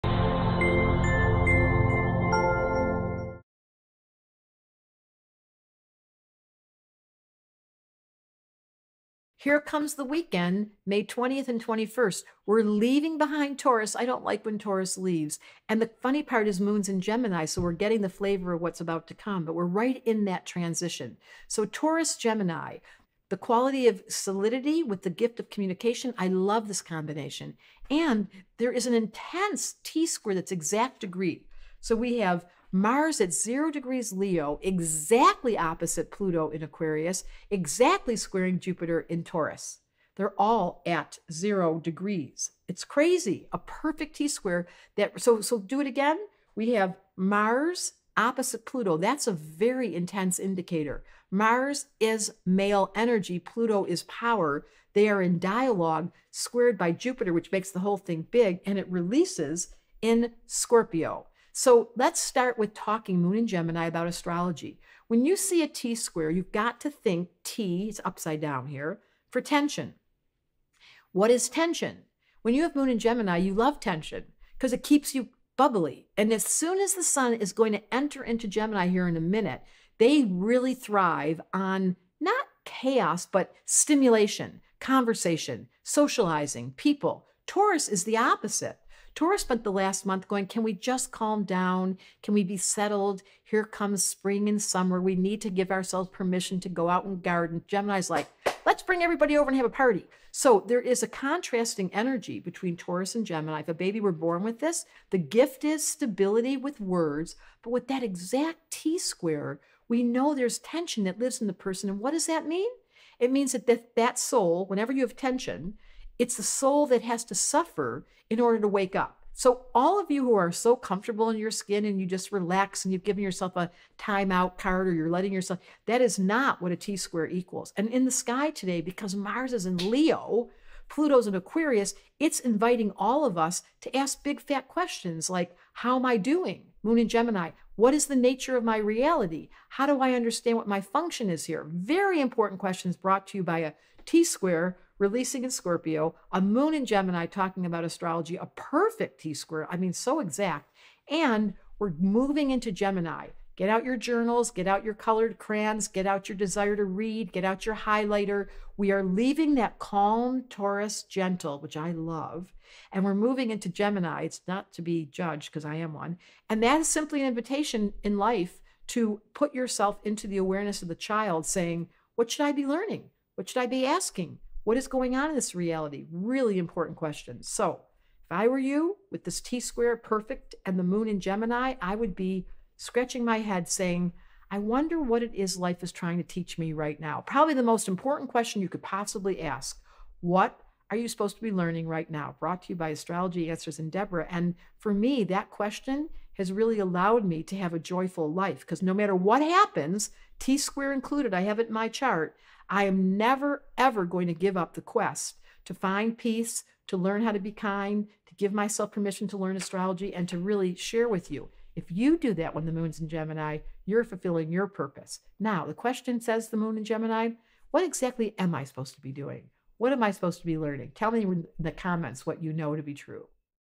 Daily Horoscope May 20th - 21st, 2023 | Astrology Answers
Are you ready for the Daily Horoscope?

The Sun is in Taurus, moving into Gemini, and the Moon is in Gemini. There is an intense T square releasing in Scorpio, bringing us the balance of Taurus and Gemini, stimulation and conversation balanced with solidarity and calmness.